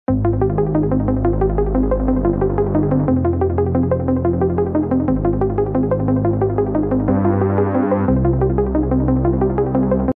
Experience the power of remote control with Neurotech Designs!
Whether you're across town or across the globe, take command of your home functions with ease.

Like and Follow @neurotechsh